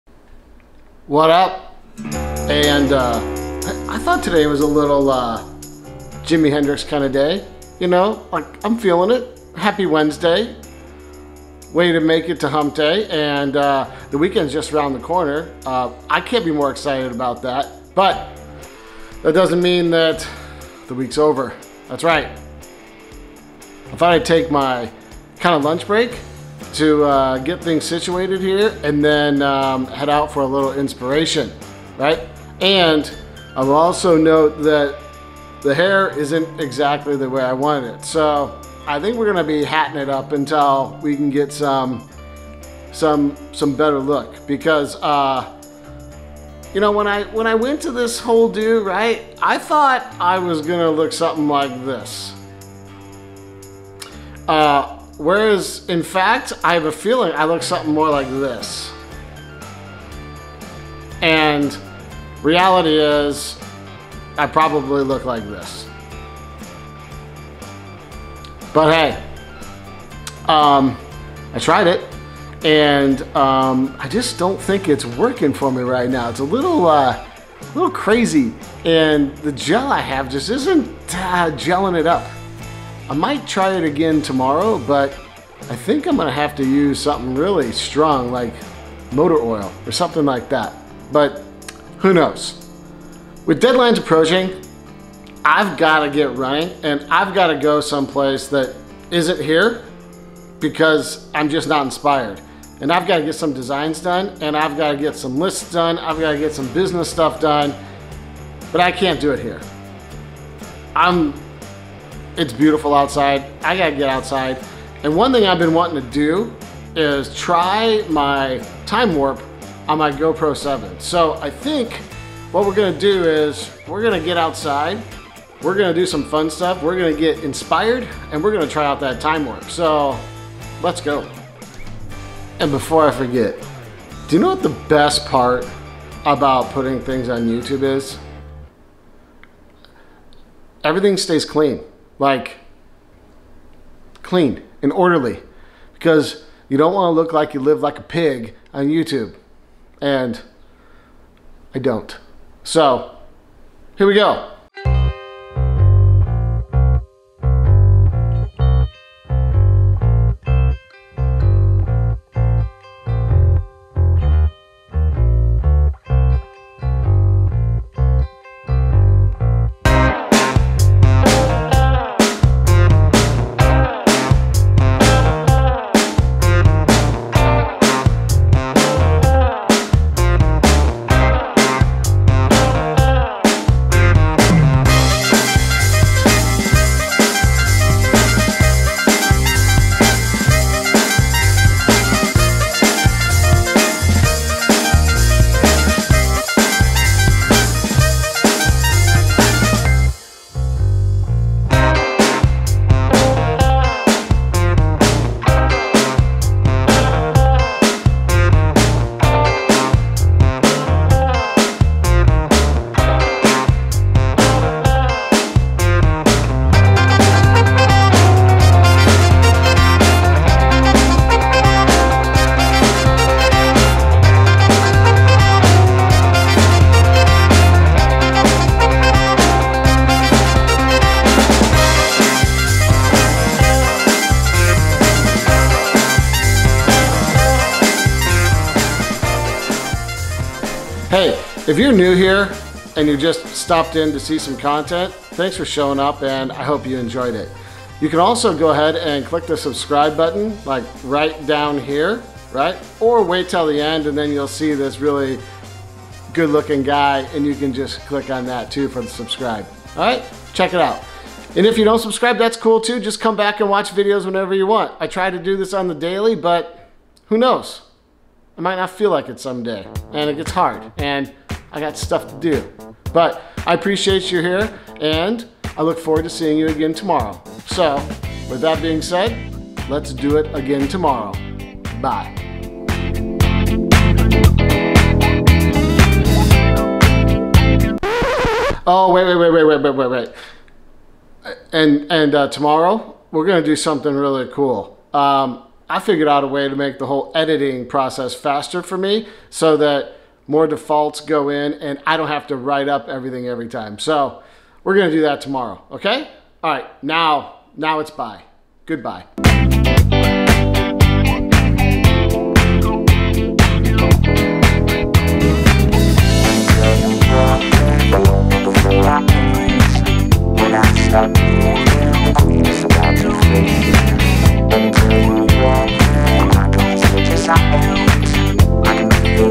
Inspiration - TimeWarp with GoPro 7 Black
I've always wanted to use the TimeWarp feature on my GoPro 7 Black. Similar to the Time Lapse option, but slightly different. The stabilization in this feature is amazing, and when used correctly in a video, it can add a great dimension to your sequencing. TimeWarp helps tell a story without sound and in a short amount of time.